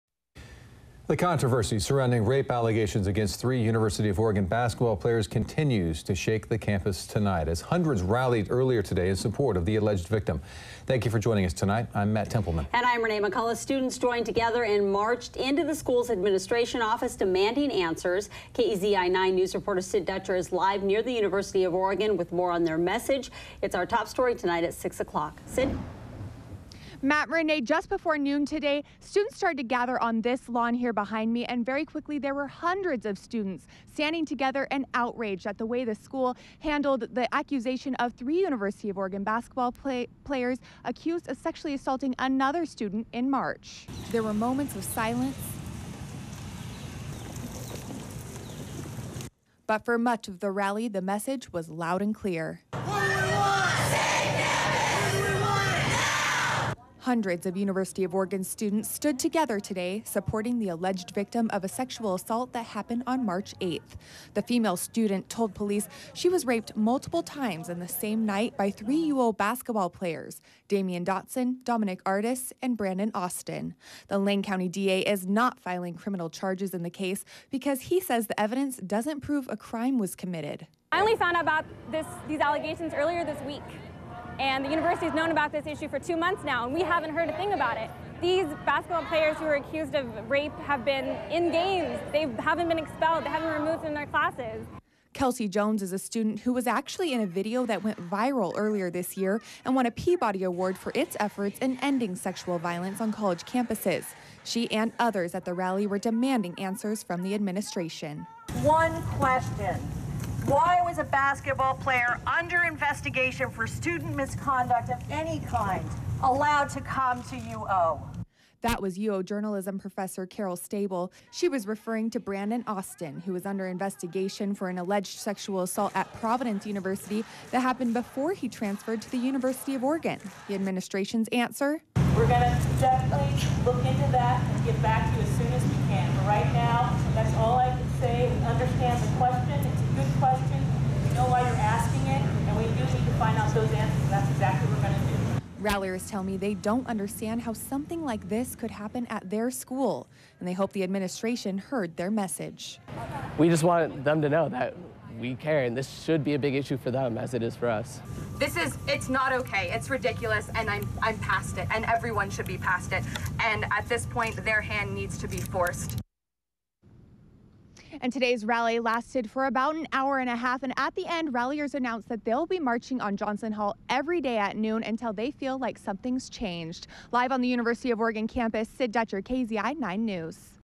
Students Rally Against Sexual Violence
Hundreds of students rallied at the University of Oregon campus Thursday, expressing their anger over how the alleged rape investigation involving three basketball players was handled.